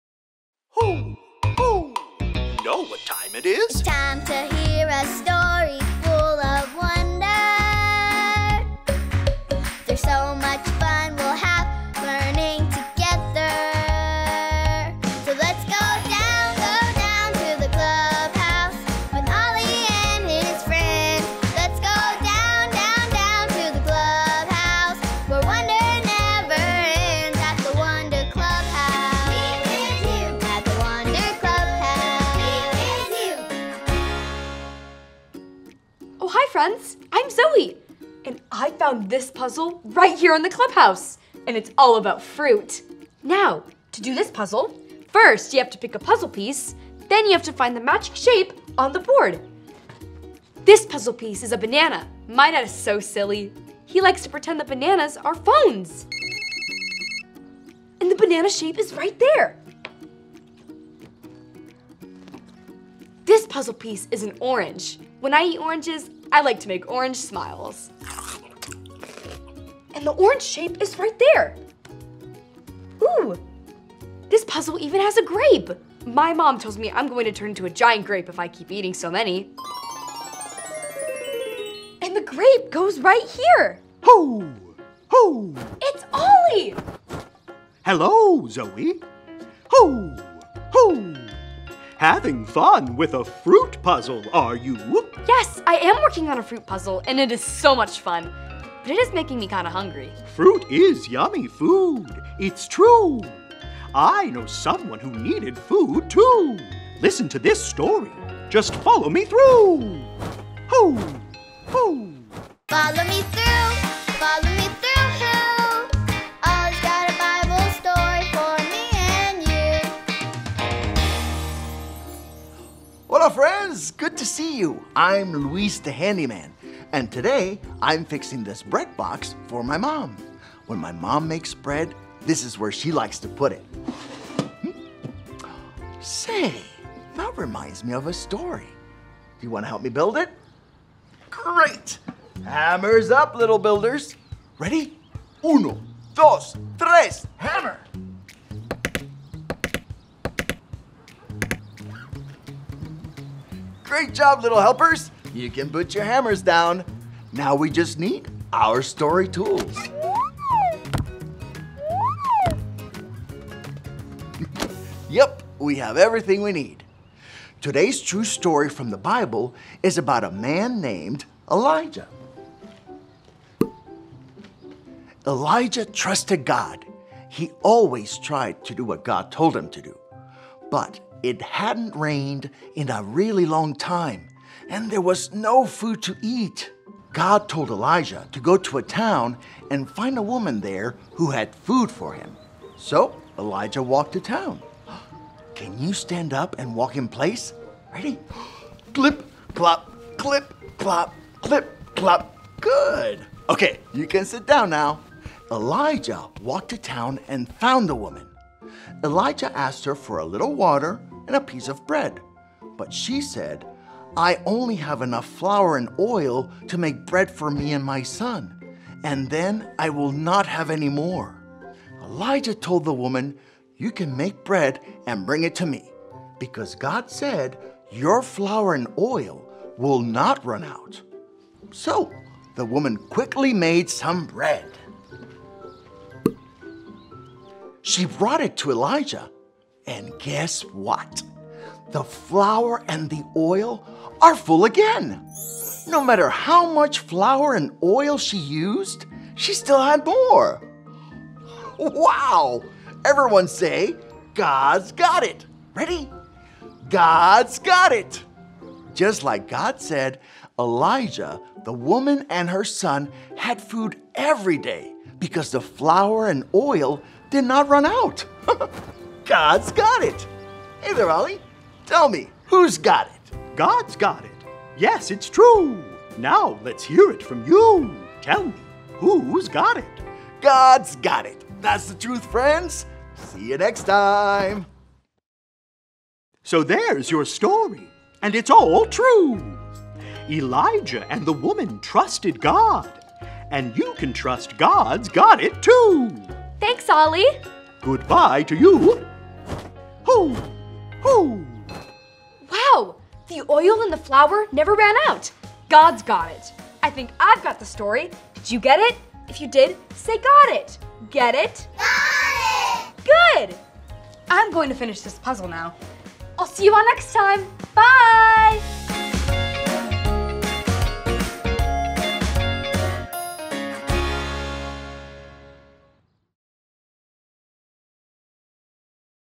November 16, 2024
Story Focus: I can trust God no matter what. Elijah
trusts God. But it hasn’t rained in a really long time and
there is no food to eat. God tells Elijah to go to a town
and find a woman there who has food for him. Elijah finds
the woman, but she only has enough four and oil to make
bread for her and her son. Elijah tells the woman that God
says her flour and oil will not run out. The woman trusts
God no matter what. And just like God said, Elijah, the
woman, and her son had food every day because the flour
and oil did not run out. Once again, God’s got it!